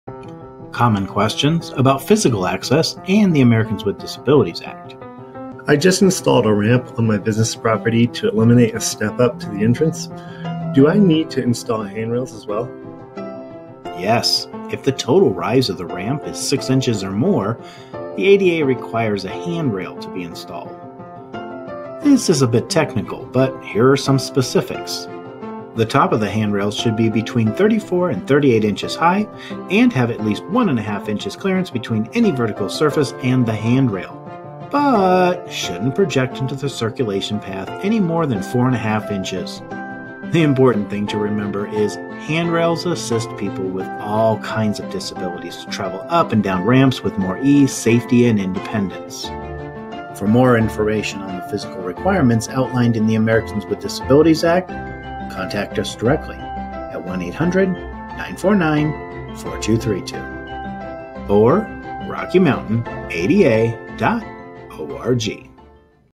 Americans with Disabilities Act - Handrails
Common questions about physical access and the Americans with Disabilities Act (ADA).  What are the requirements for installing handrails along ramps?